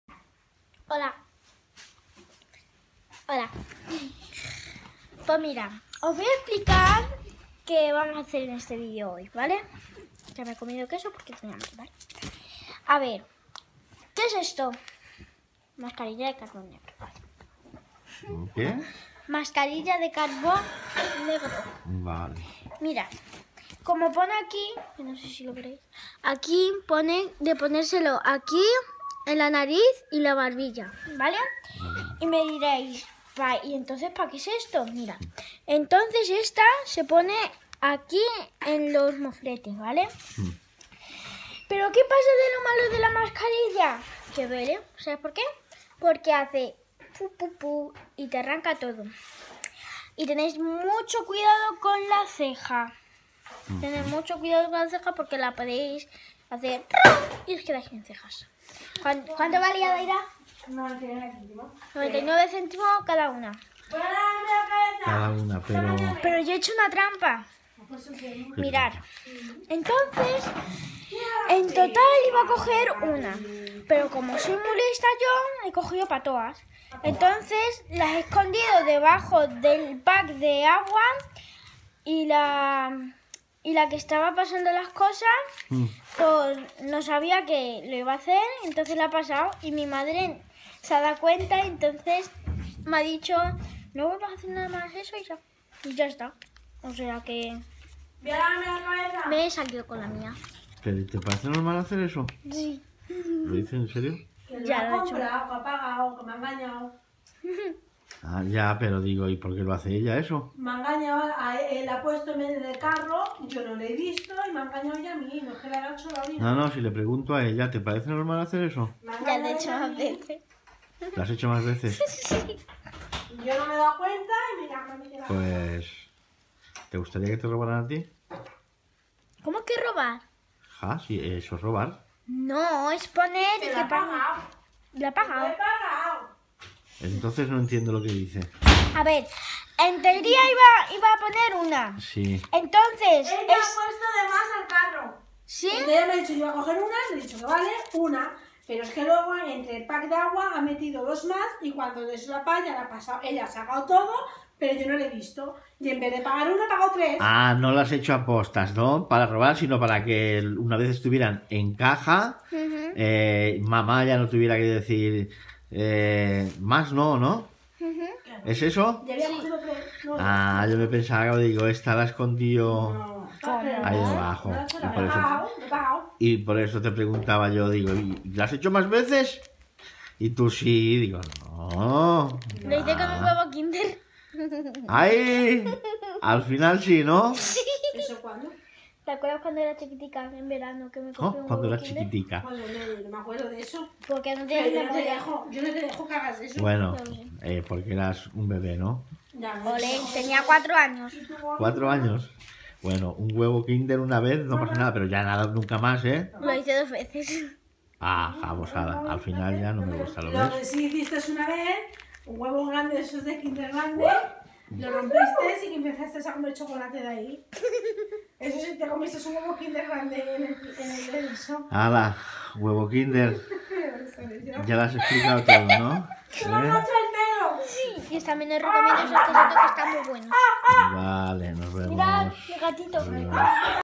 Mascarila de carbón negro 😂😂
Que lio con la garrafa y la mascarillas. Saludos